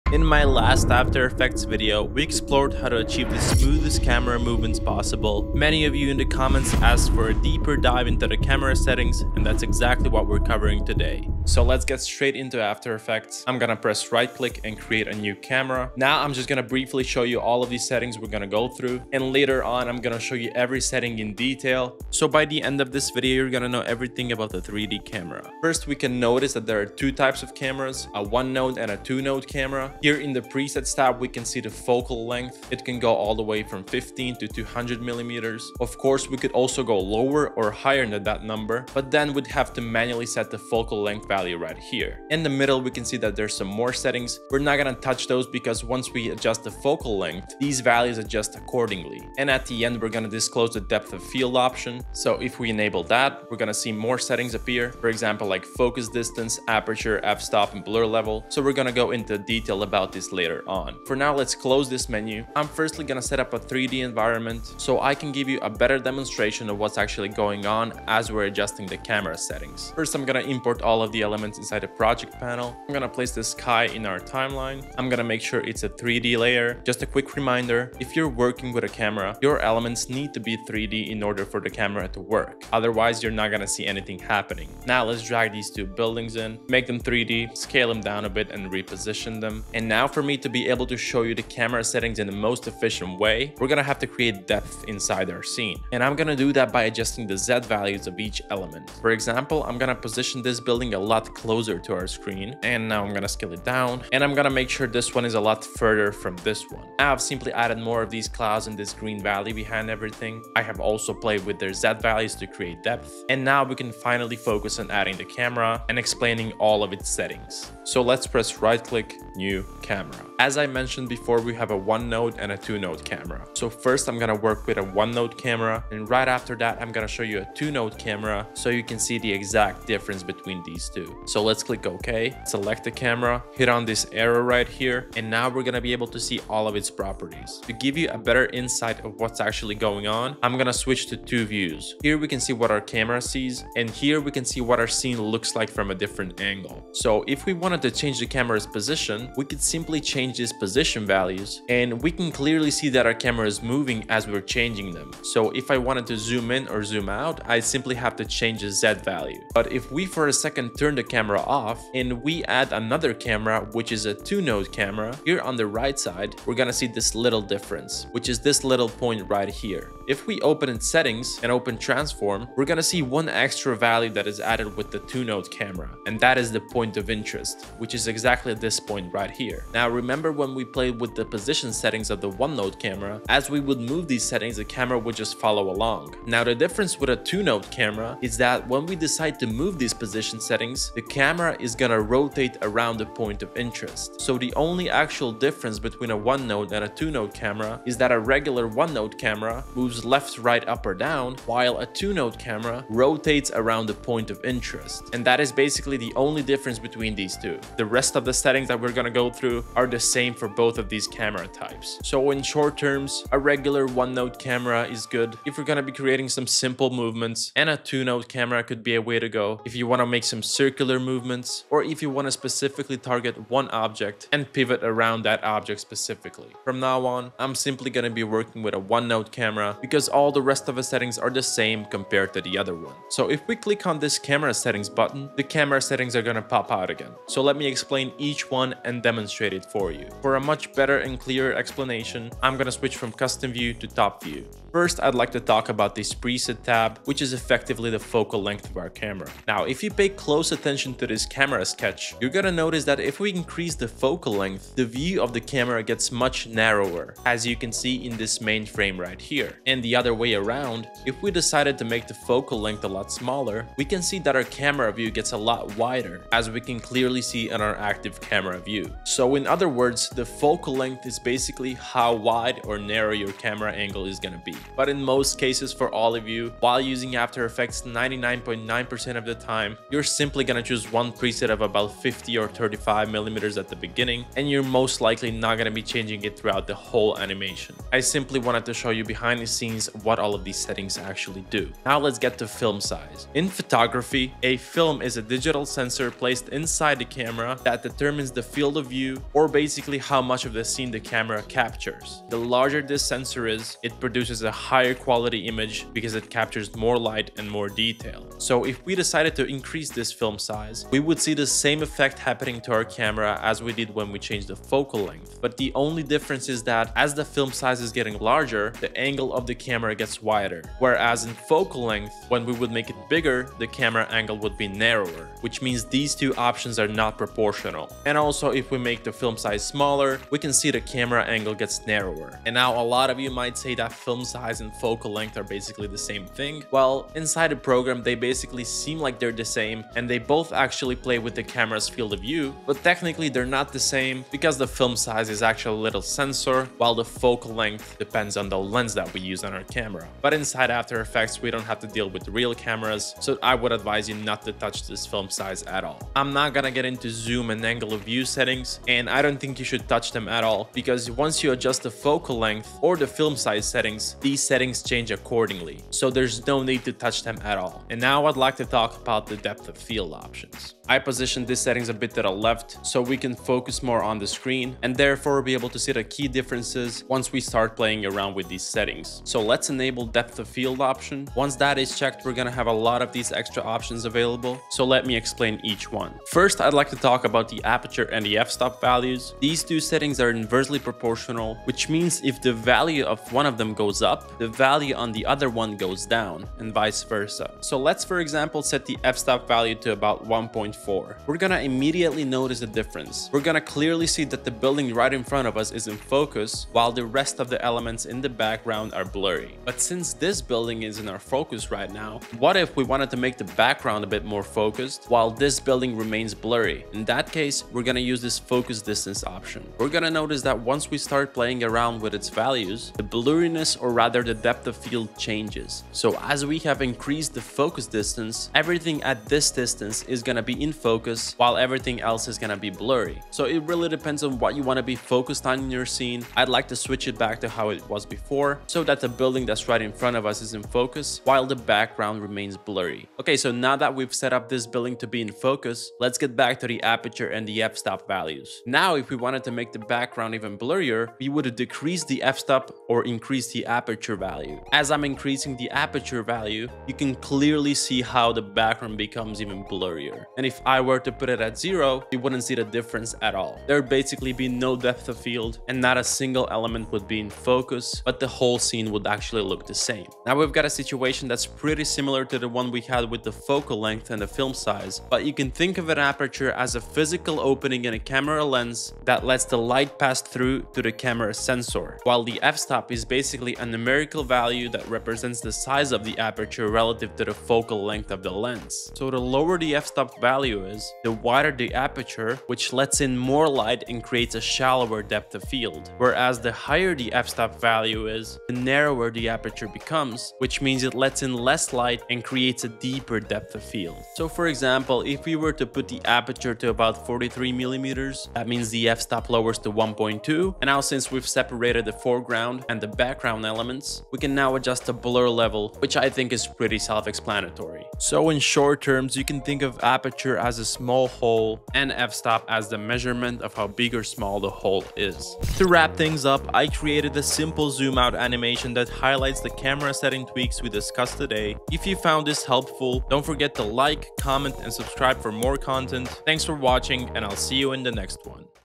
Camera Settings Explained - After Effects Tutorial
Unlock the full potential of After Effects with this in-depth guide to mastering camera settings. In this video, I'll break down every element of the After Effects camera, from focal length to depth of field, and explain the theory behind each feature.